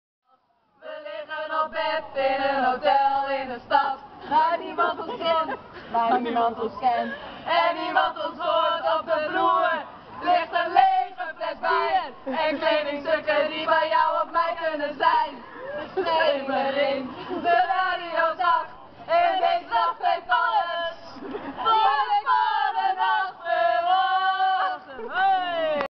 Sex in the City - Vrijgezellendag voor Vrouwen spel in Sneek op 15-08-2015 (nr. 25)
Op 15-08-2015 was het Sex in the City - Vrijgezellendag voor Vrouwen spel in Sneek als CityGame, BachelorParty, ThemeParty of Company game